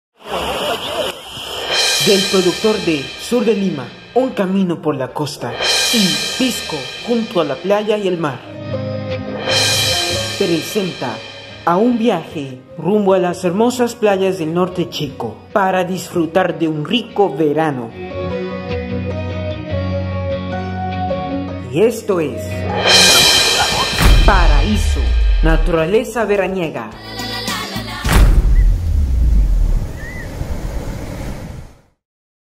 ☀️ PARAÍSO: NATURALEZA VERANIEGA 🏖️ - TRAILER 2023 4K 🕶️
🏖️ Muy buenas tardes, amigos. Hoy día tenemos el primer trailer del próximo documental del año 2023. Se trata de una playa que se ubica a un par de horas desde Lima, nos referimos a Playa Paraíso. A unos metros cerca de la ciudad de Huacho. El cual posee unas olas tranquilas, el rico sol, y por su puesto, una bellísima albufera dónde habitan muchas especies de aves. Un recorrido maravilloso por la fascinante playa del norte chico para disfrutar de un increíble verano. ☀️🇵🇪